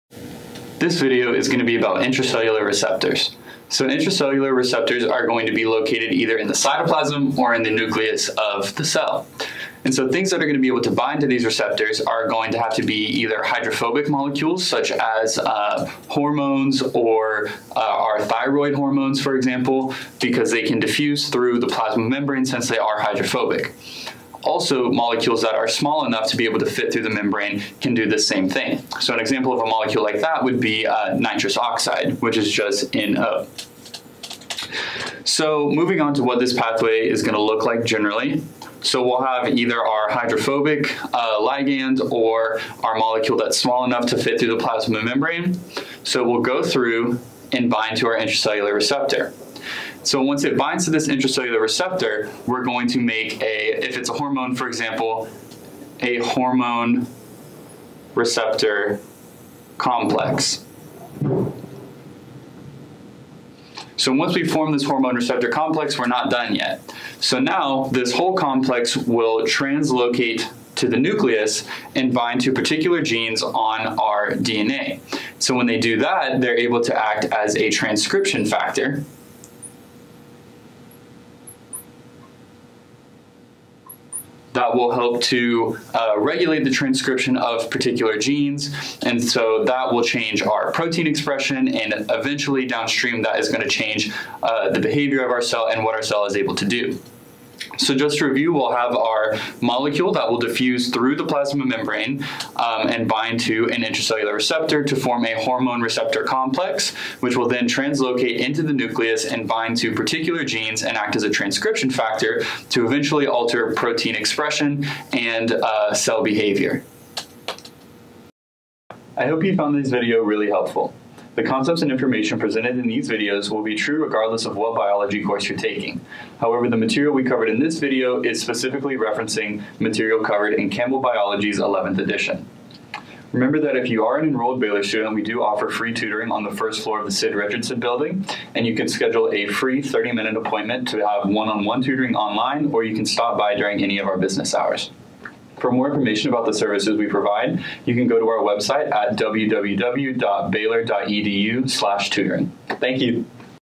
Intracellular Receptors
This video explains the structure and function of receptors found in the cytoplasm of the cell, and how they are fundamental for many cellular processes activated by different molecules.